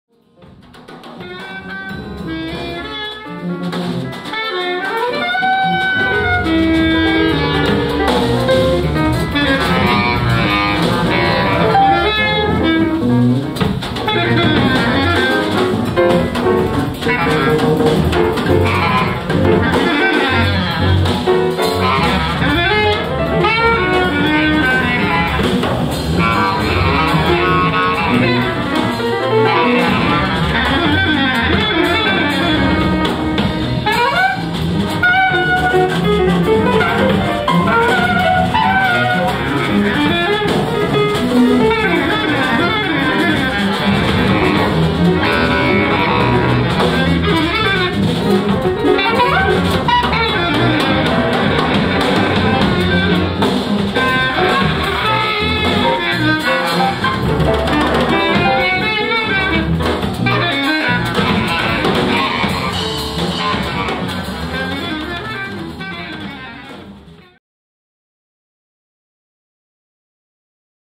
Utrecht Jazz Fest Red-trio+JasonStein 11 maart 2010.
Red-trio+JasonStein Utrecht Jazz Fest 11 maart 2010.
Rodrigo Pinheiro - piano, geprepareerde piano

Hernâni Faustino - contrabas

Gabriel Ferrandini - drums, percussie

Jason Stein - basklarinet